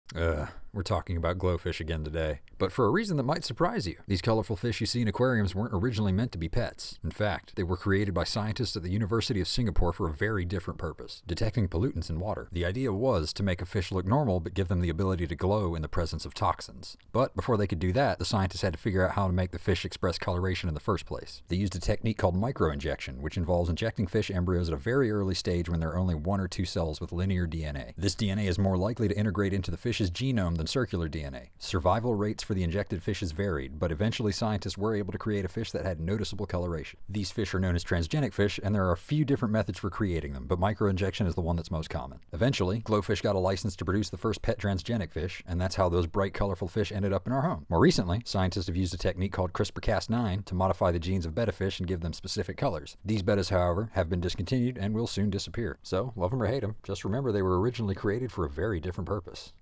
Minute Science: Transgenic Fish
We've talked about GloFish, but they weren't originally made for aquariums...